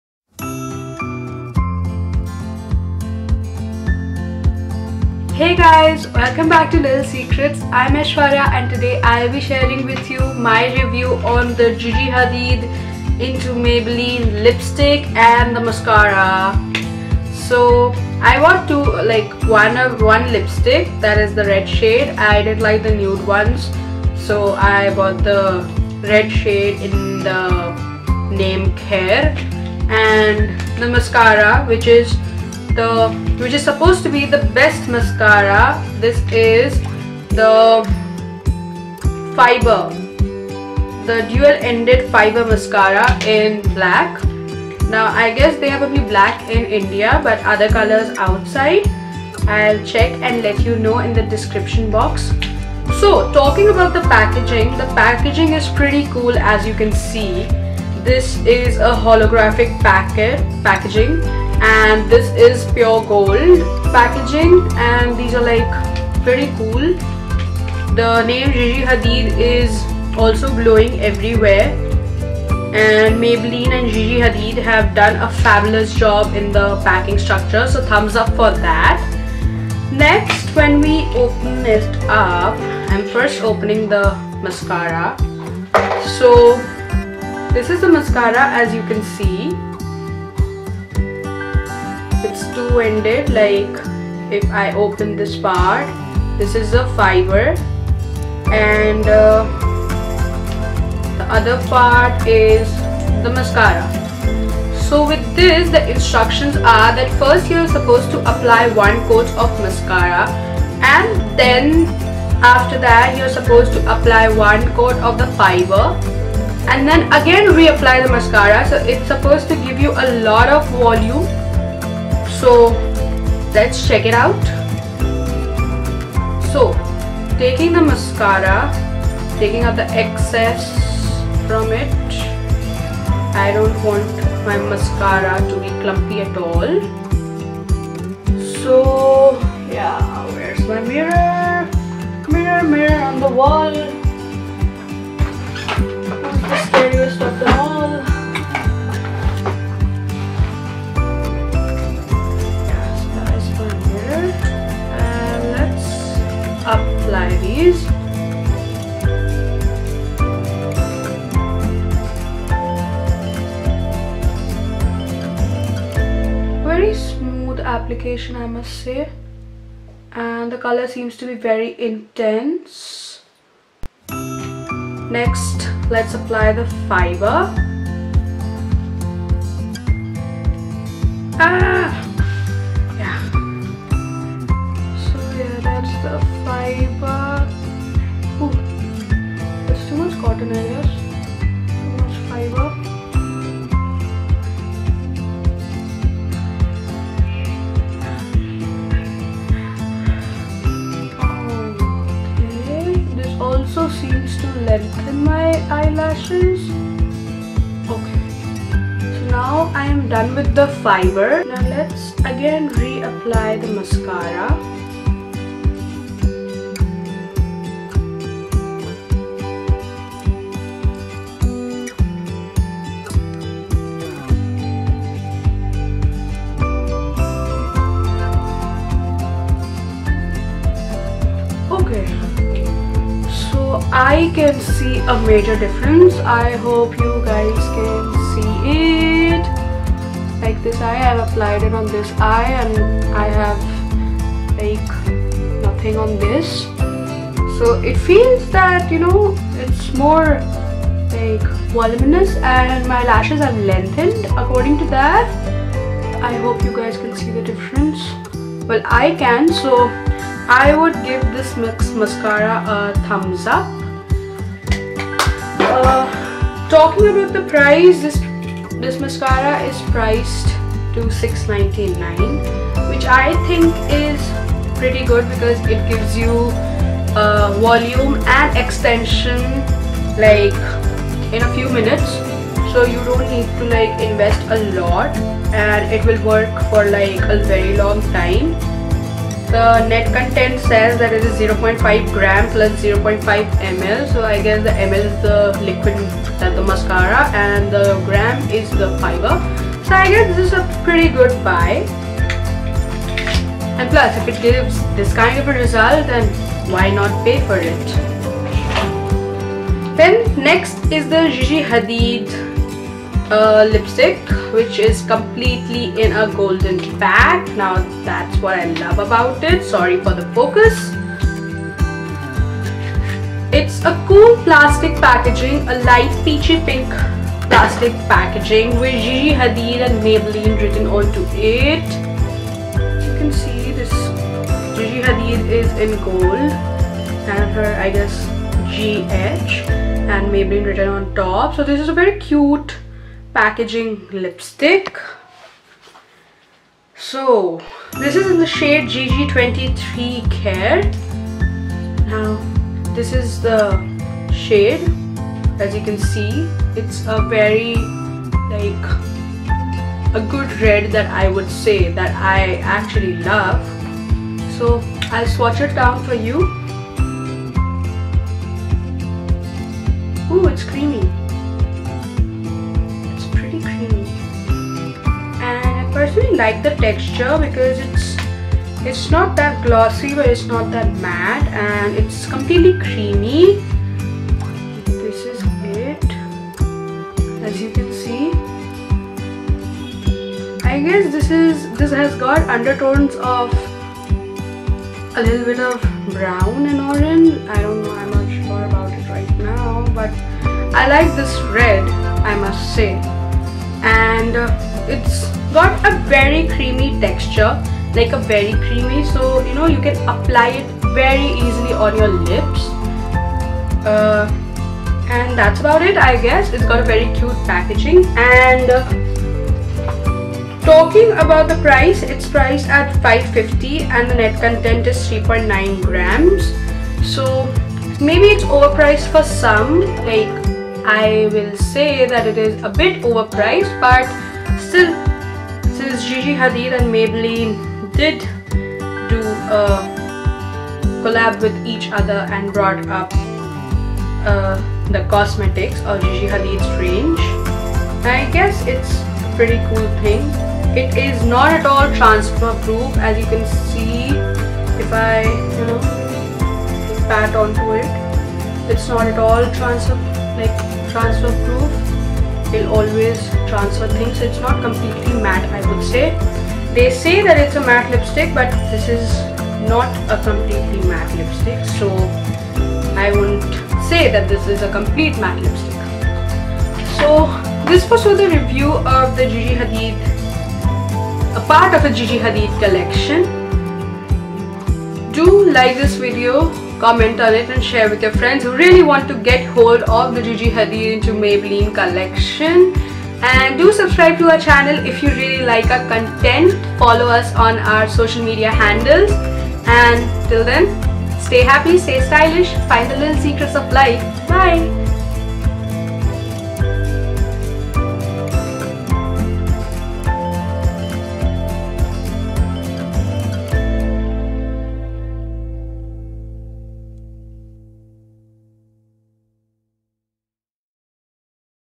GigiHadidXMaybelline Product Review + Live Testing/Demo - Little Secrets
Product review || Gigi Hadid X Mybelline || Gigi Hadid || Maybelline product review || Mascara || Lipstick

Check out the latest launched product reviews for the Gigi HadidXMaybelline right here ⬆️

The two products mentioned are -
GigiHadidXMaybelline Matte Lipstick - Khair
GigiHadidXMaybelline Fibre Mascara - Black